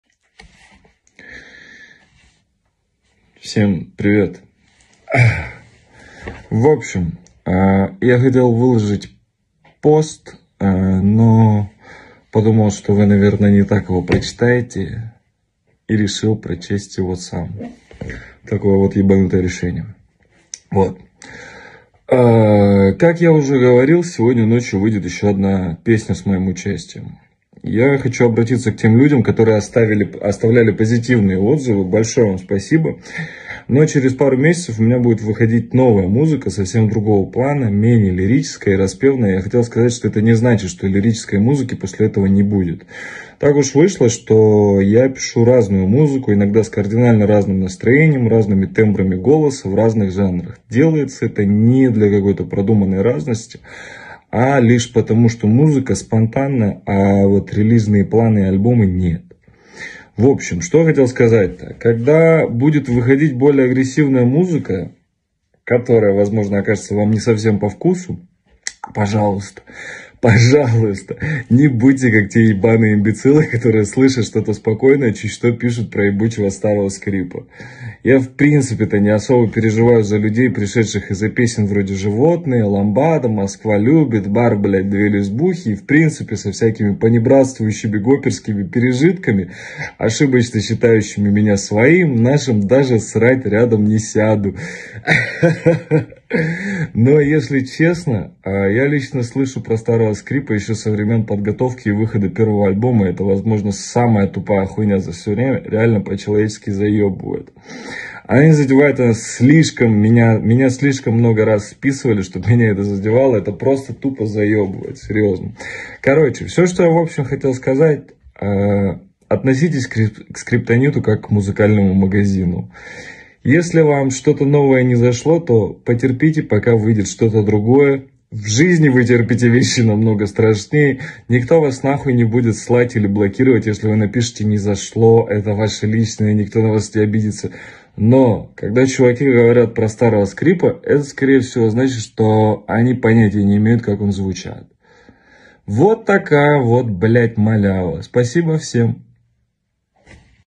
Музыка Скриптонита в 2023 будет жестче: готовьтесь к новым релизам
В этом видео мы разбираемся с тем, что нас ждет от рэппера Скриптонита в 2023 году. Он известен своей жесткой музыкой и решительным стилем рэпа, и мы ожидаем, что его новые релизы будут еще более мощными. Мы также обсуждаем то, что можно ожидать от Скриптонита в будущем, и как относиться к нему как к музыкальному магазину, то есть к исполнителю, который всегда готов порадовать своих слушателей чем-то новым и эксклюзивным. Если вы являетесь фанатом Скриптонита или просто хотите узнать больше о его музыке, то это видео обязательно для вас.
-periscope Oxxxymiron
-periscope баста
-periscope Kizaru
-periscope Schokk
-periscope Face
-periscope Kizaru